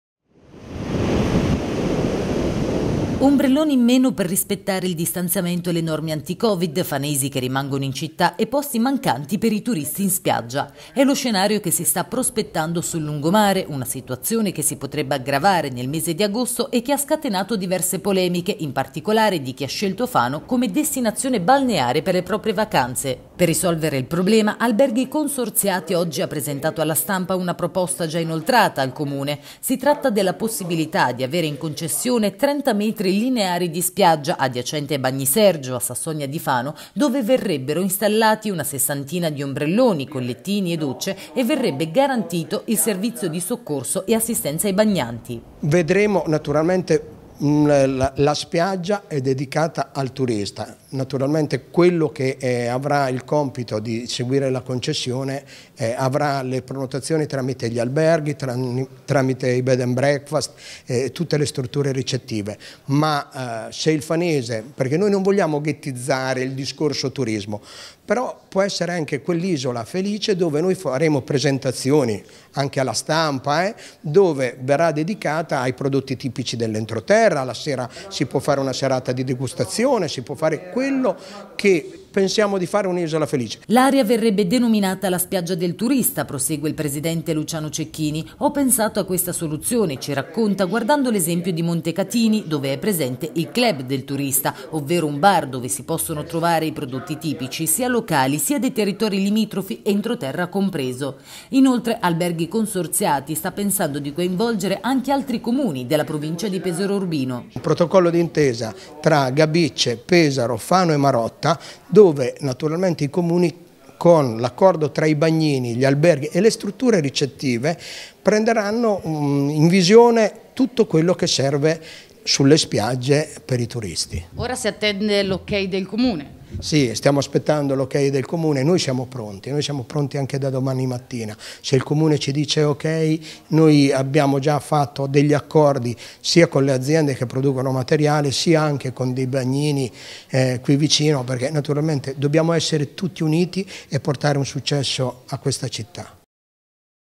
Alberghi Consorziati chiede concessione spiaggia per turisti
A Fano mancano gli ombrelloni per i turisti: Alberghi Consorziati chiede al Comune una spiaggia in concessione.